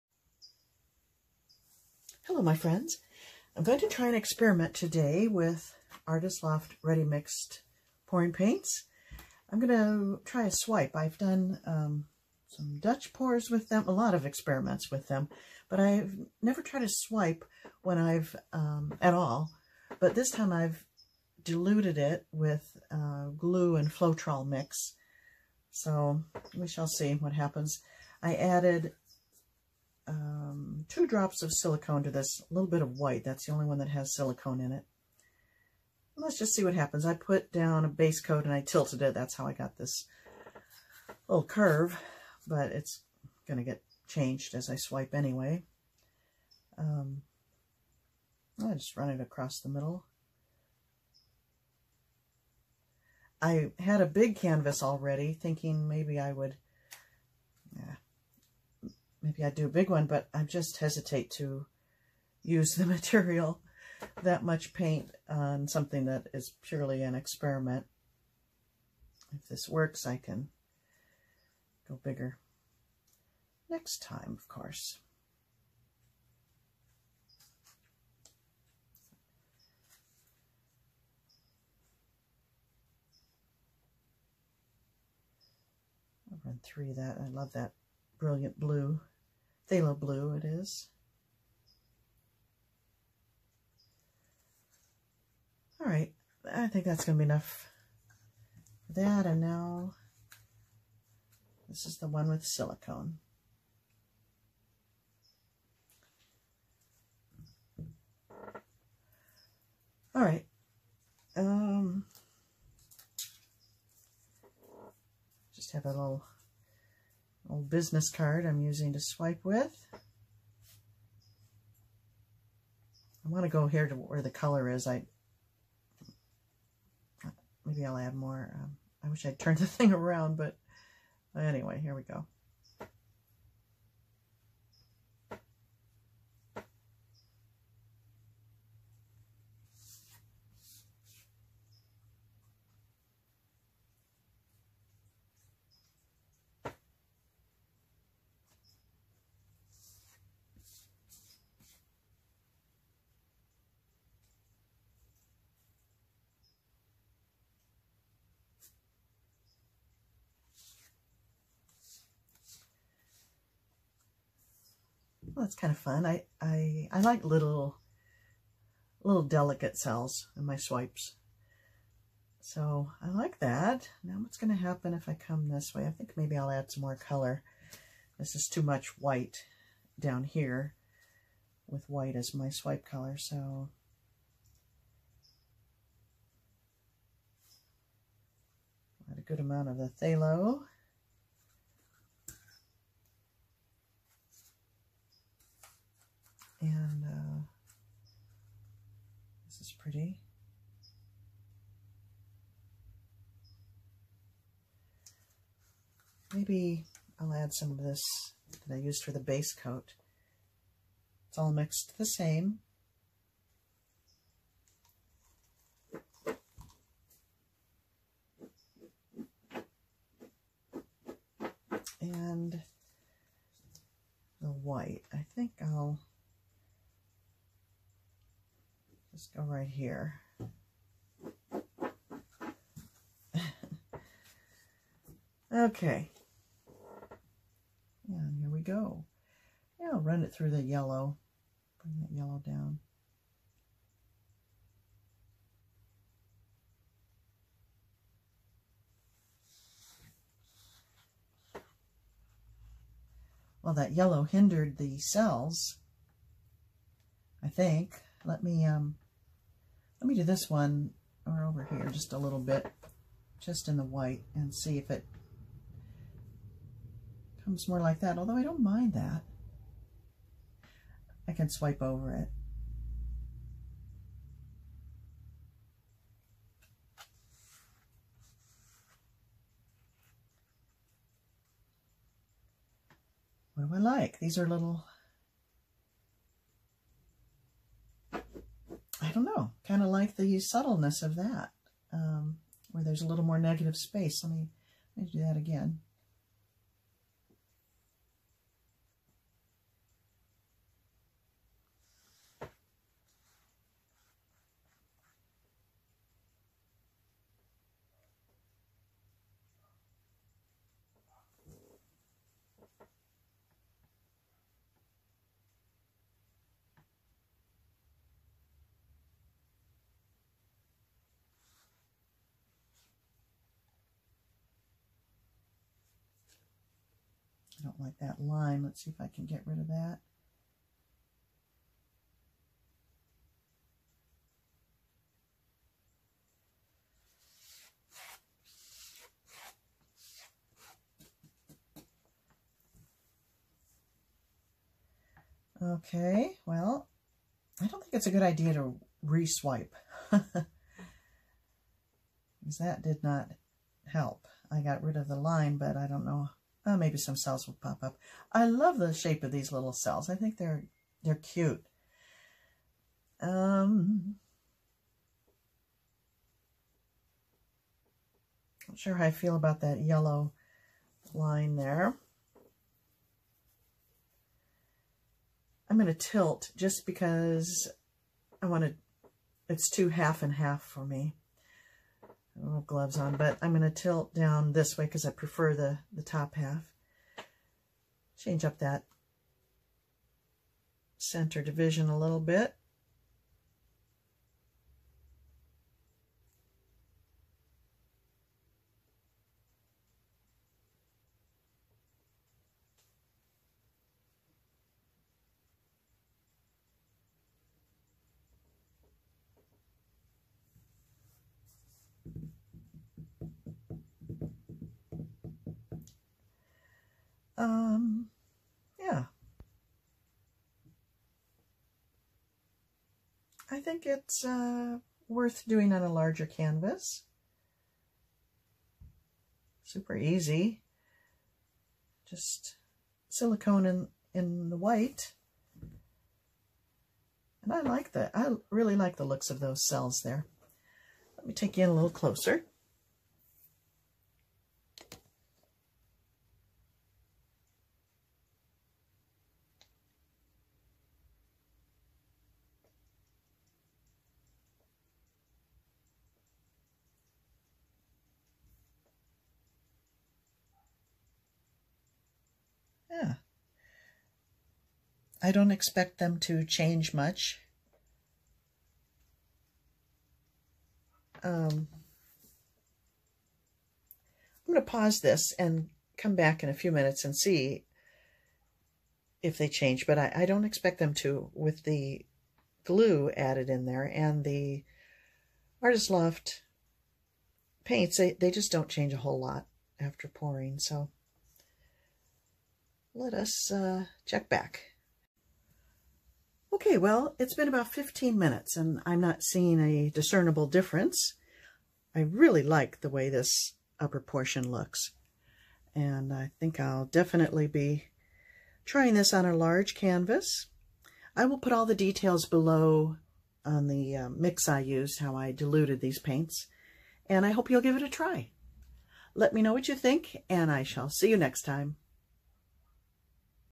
(94) ACRYLIC POURING SWIPE - Using Artist's Loft Ready Mixed Pouring Paint - SILICONE in one color
Colors used: Artist's Loft Ready Mixed in White, Turquoise mixed with white, Aqua, Warm Yellow and Phthalo Blue.  All paints were mixed one part paint and one and a half parts of the following pouring medium:

Pouring Medium:
5 Parts Floetrol
2 Parts Elmer's Glue All

Two drops of silicone were added to the white swipe color only.